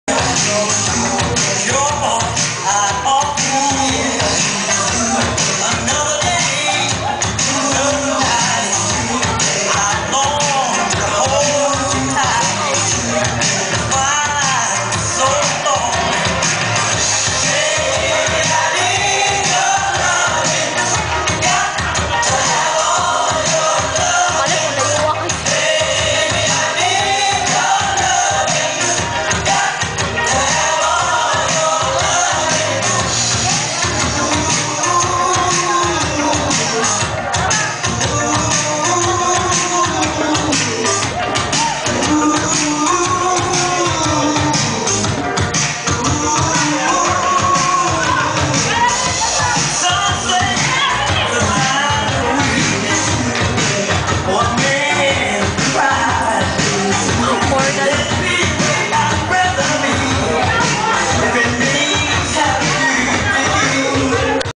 tango in dumaguete
bored. so i went dancing almost every night =)